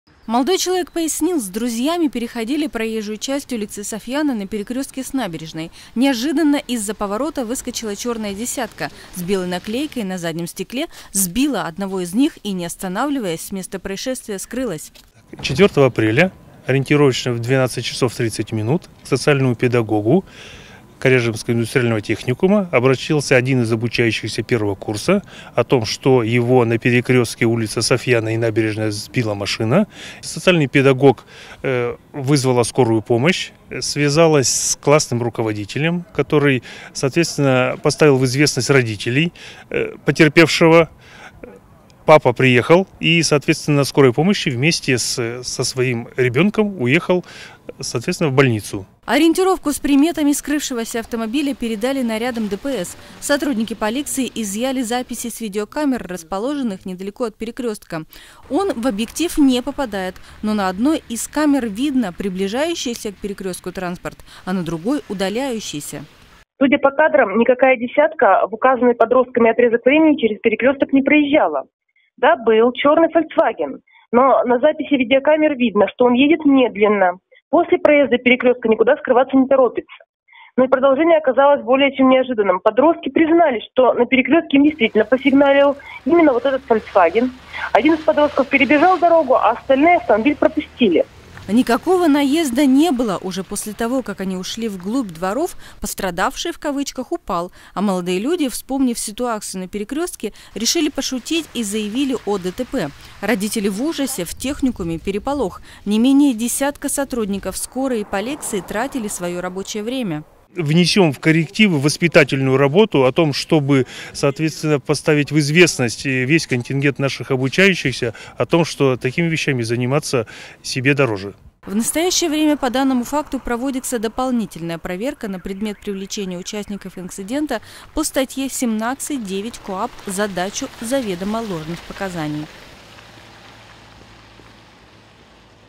Ложное сообщение о ДТП
4 апреля в отдел полиции  поступило сообщение о ДТП, в котором пострадал 16-летний юноша. Молодого человека на Скорой отправили на обследование в поликлинику. Сотрудники ГИБДД приступили к розыску автомобиля.